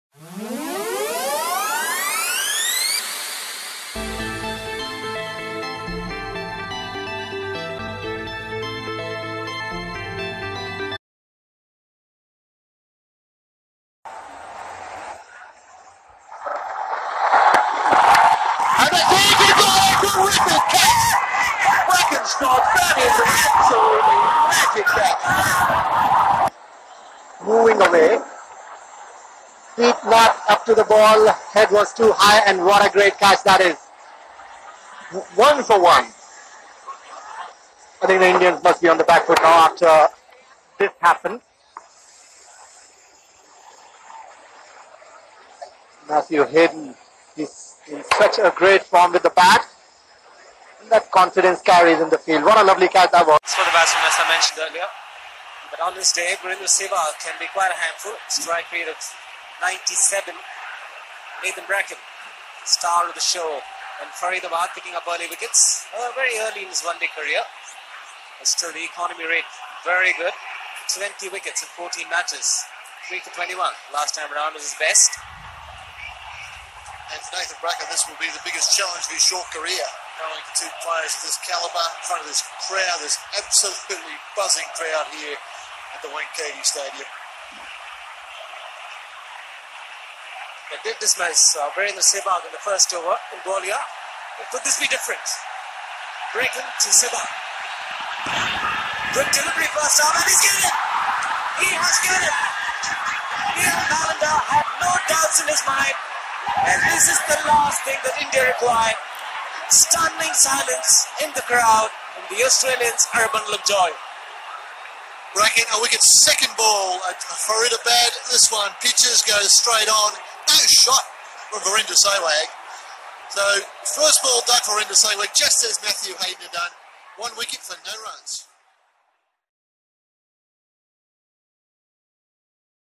Sehwag duck out 2 continuous matches dismissed by Nathan Bracken.
The famous Aussie bowler Nathan Bracken dismissed sehwag at 0 runs in the same series next to next match on TVS cup Tri series 2003.